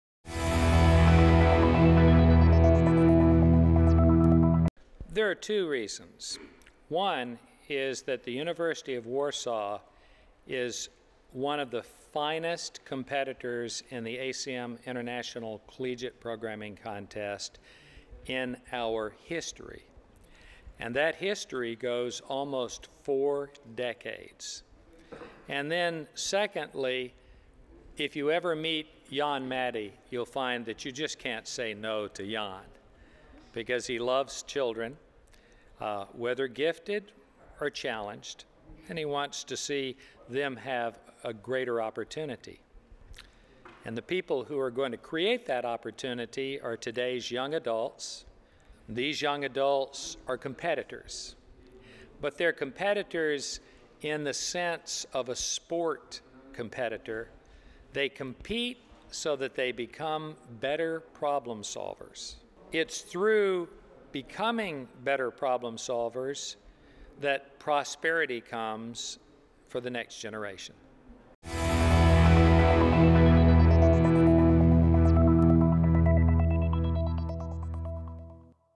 Interview with prof. Bill Poucher (ACM ICPC Executive Director, Baylor Universtiy)
Interview with prof. Bill Poucher (ACM ICPC Executive Director, Baylor Universtiy) during The 2011 Polish Collegiate Championship in Team Programming (29-30 October 2011).